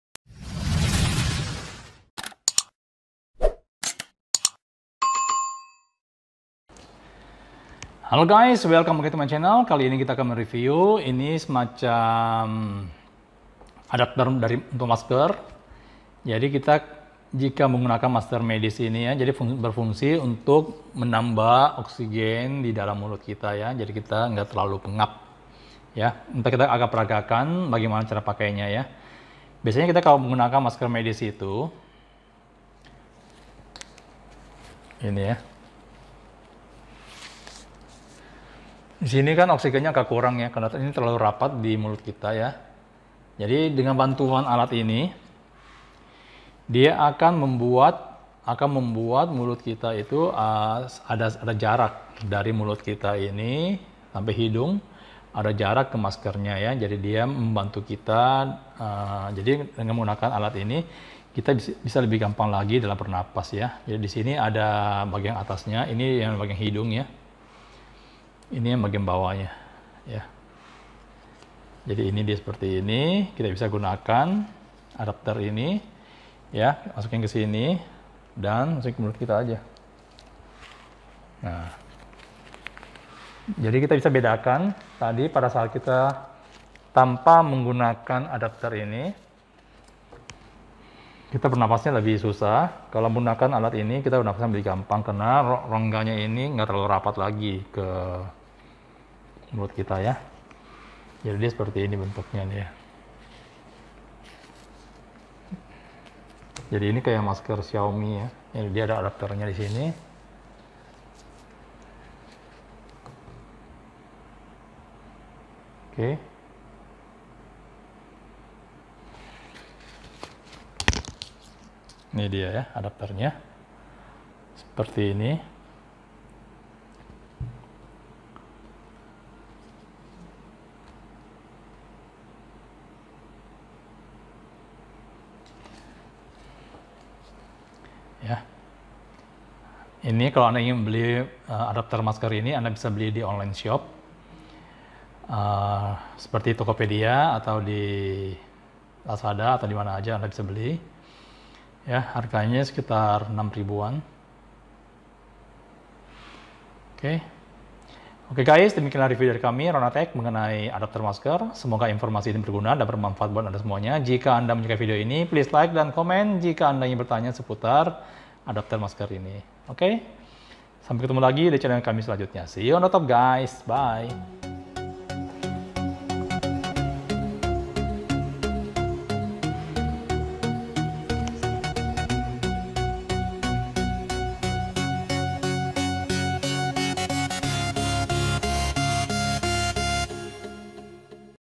Ini Rahasianya Biar Nafas Lega Saat Pakai Masker.
Deskripsi Masker Penyangga Bracket Masker 3D Support Penahan Elastis N95 Medis
Ready Stock siap kirim.
Bukan PO
*Merasa pengap dan tidak nyaman saat pakai masker ?
*Mulut sering sentuh masker sehingga basah ?
*Gak bisa pakai lipstick dan makeup ?
Ginakan:
Masker Penyangga Bracket Masker 3D Support Penahan Elastis N95 Medis
Keunggulan :
Anti Pengap & Sesak
Masker lebih awet & tahan lama
Nafas mudah & nyaman meskipun menggunakan N95
Ringan dan nyaman di gunakan Bahan Silikon tidak sakit di wajah
Bebas pakai lipsitck & makeup
Bisa dicuci & pakai ulang